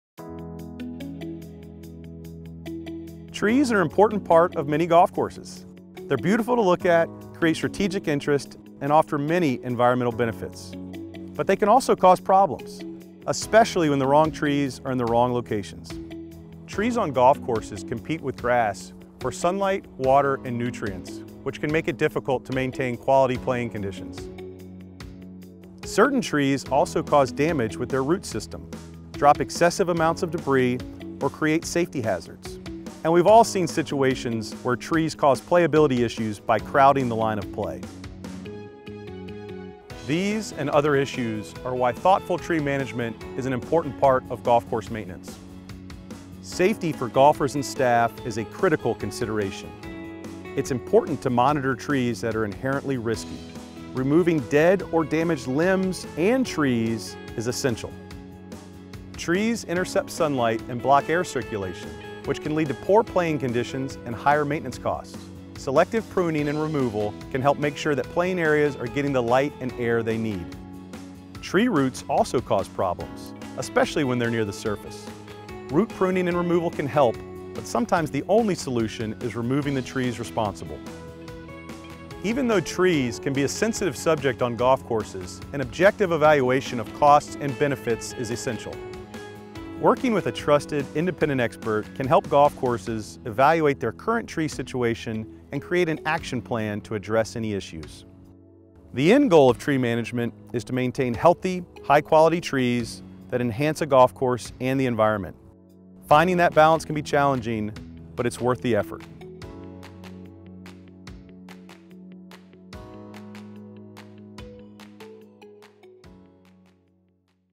Managing Trees on Golf Courses
Trees are an important part of many golf courses, but they can also cause a wide range of problems. This is why thoughtful tree management is an essential part of golf course maintenance.